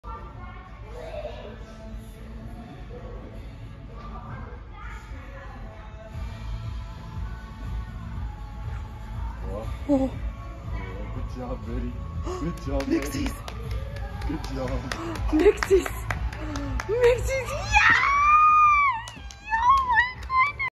He Did It!
Mixies first time waking across the balance beam! We go to open gym where Lemon takes her gymnastics class.